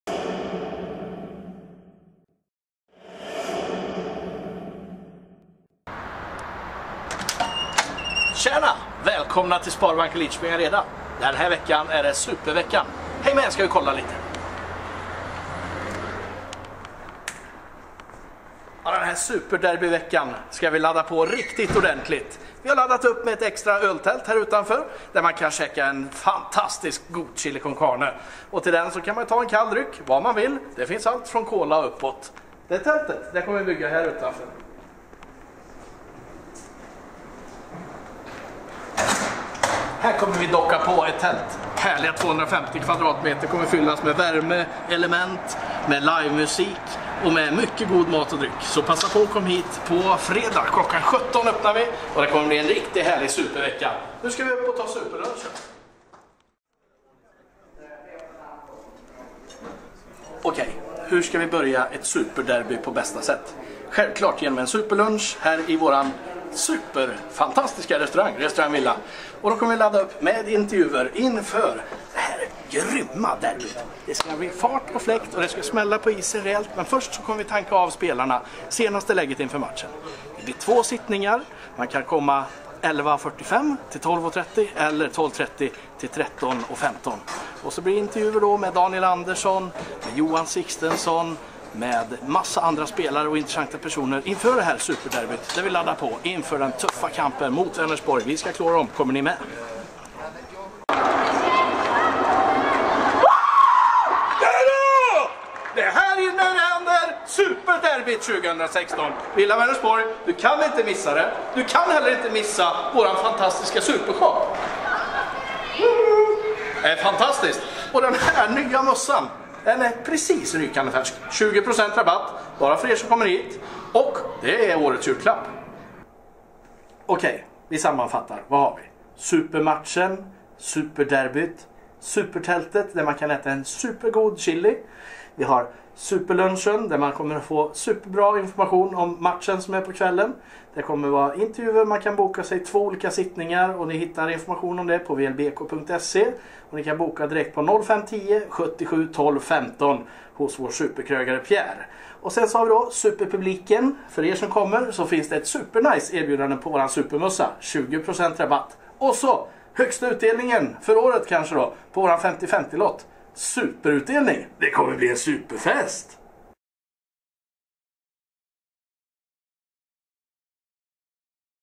INFÖR SUPERDERBYT!
Lyssna på vår supertaggade klubbchef som går igenom SUPERDERBYT mot IFK Vänersborg som spelas på fredag.